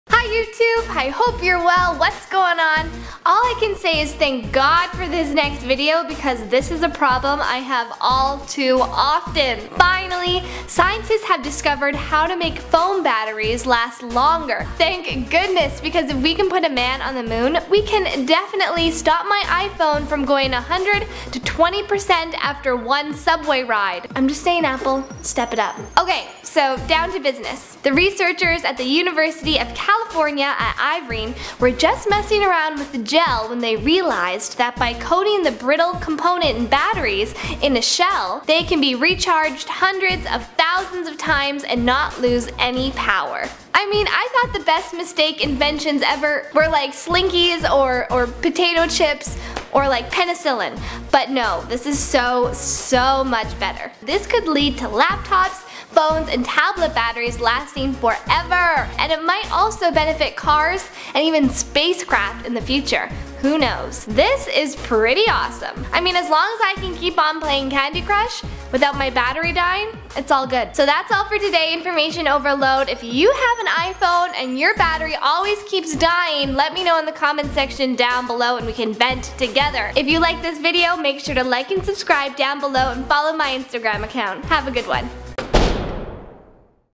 Scientist Discovers How To Make Phone Batteries Last Longer
Scientists have accidentally discovered a simple trick that could triple the life of batteries in phones, laptops, tablets and maybe even cars!

2408 Lakeshore Blvd W.

VIDEO EDITED BY:

Alan Smithee
Danica Oliva
Ryan Wazonek
Wilson Huynh
Imo Scrimger